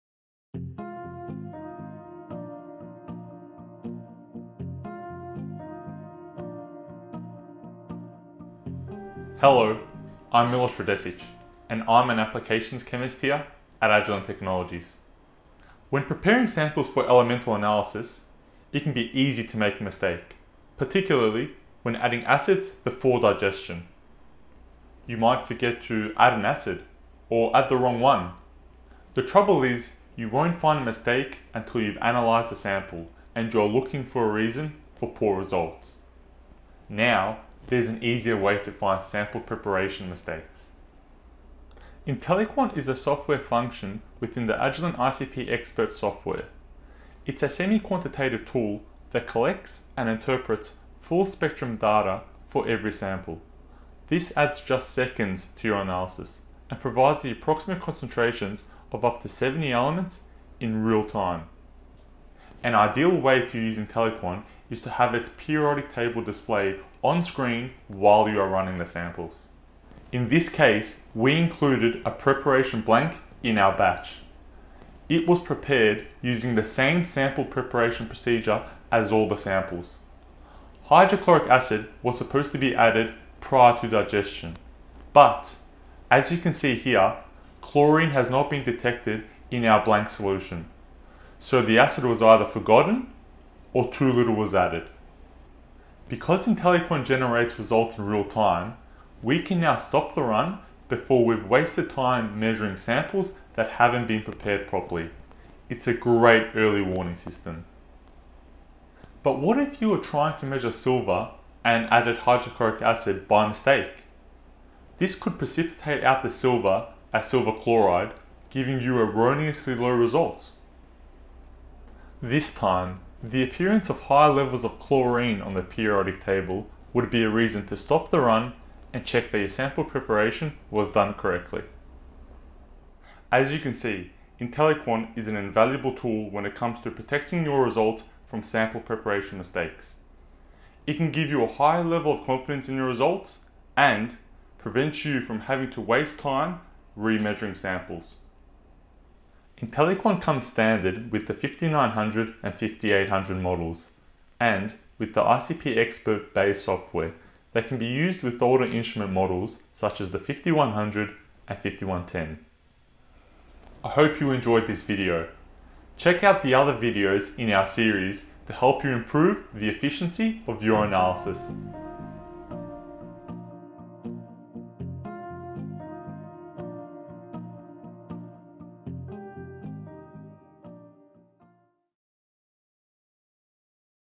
ICP-OES Sample Preparation - How can I check for sample preparation mistakes?
How to spot sample preparation mistakes before they ruin your results.